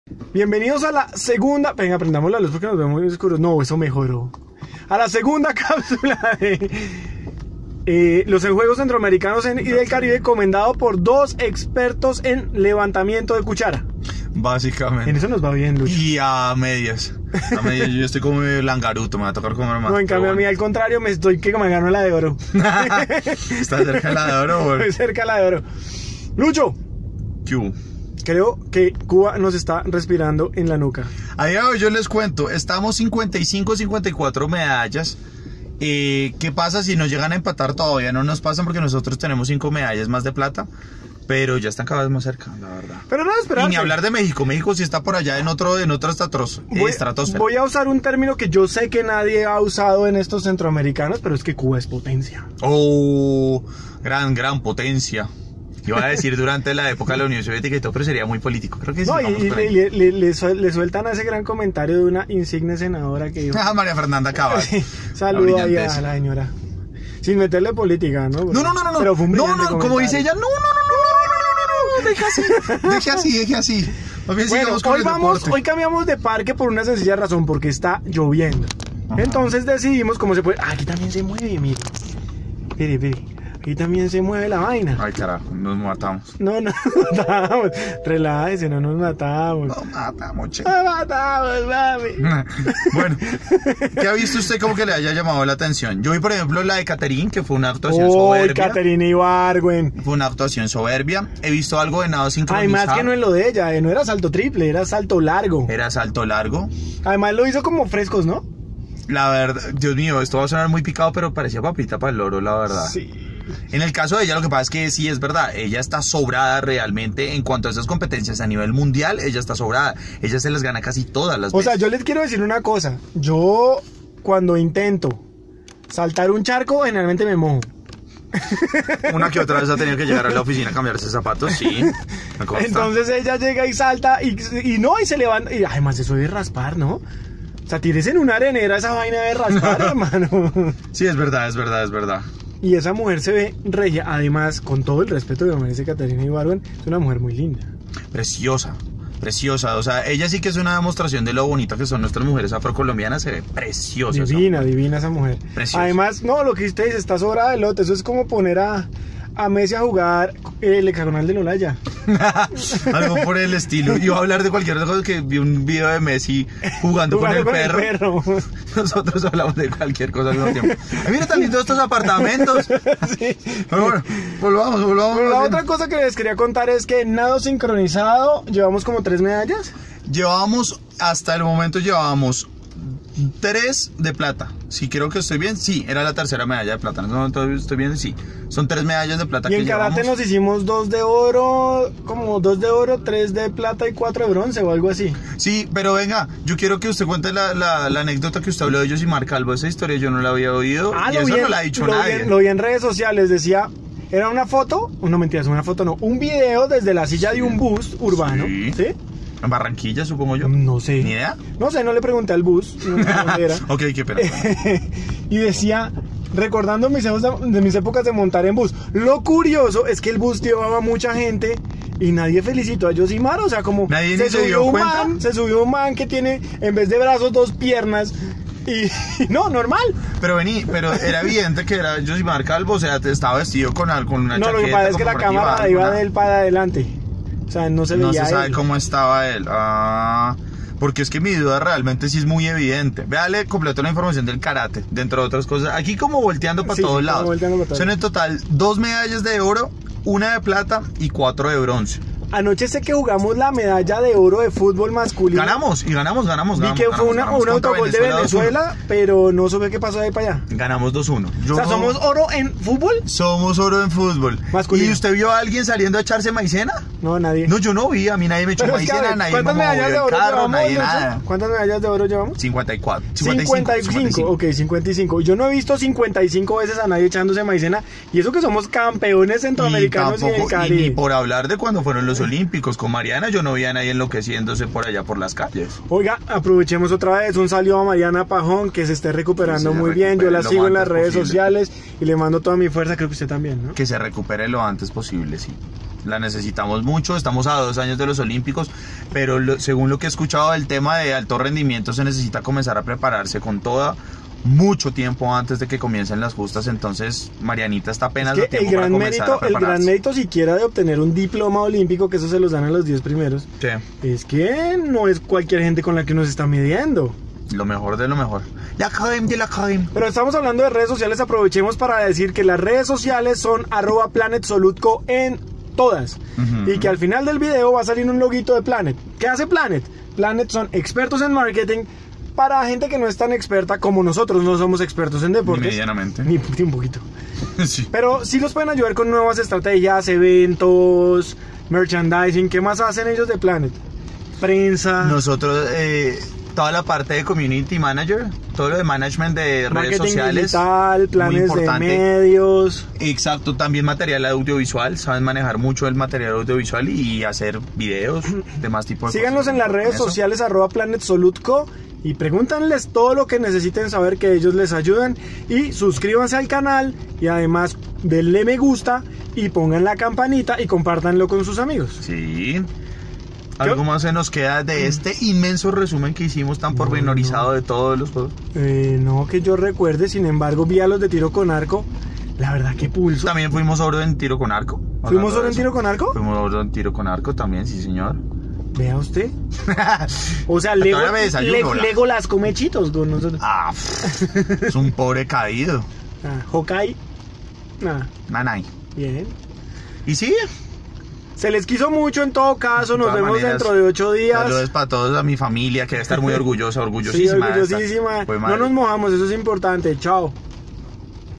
Planet en los centroamericanos - Cap 2
Segundo resumen de los Juegos Centroamericanos y del Caribe, que se disputan en Barranquilla